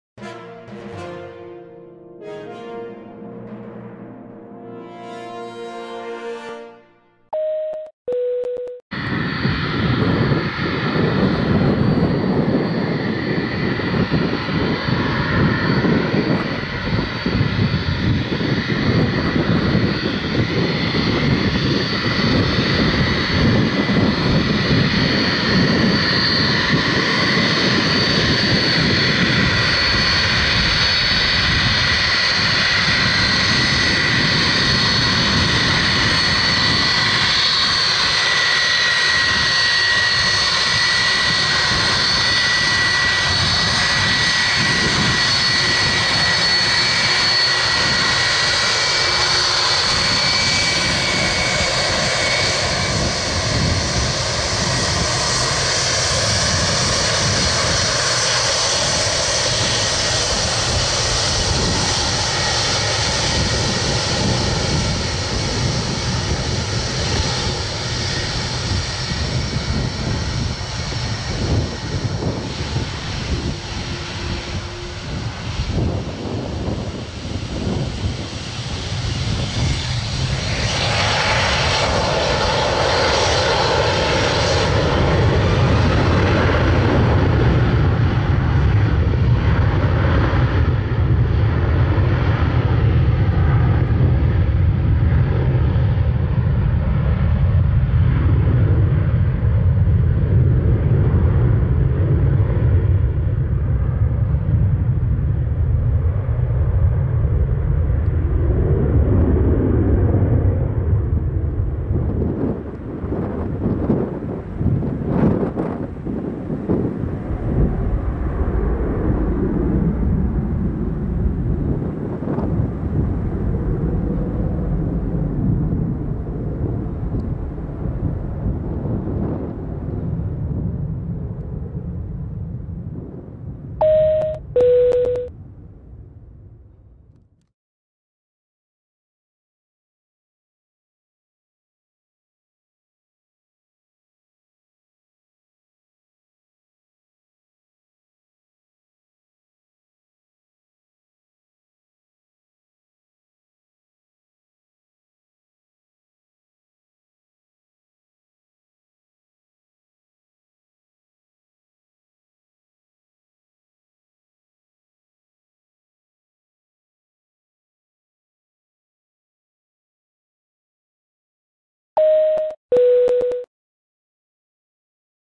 G-JMCE Thomas Cook Airlines Boeing 757-200 TAKEOFF ACE (LANZAROTE) [FULL HD]
Aeropuerto de Lanzarote-Arrecife Aeródromo Militar de Lanzarote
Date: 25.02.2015
Camera:Sony HDR-CX900E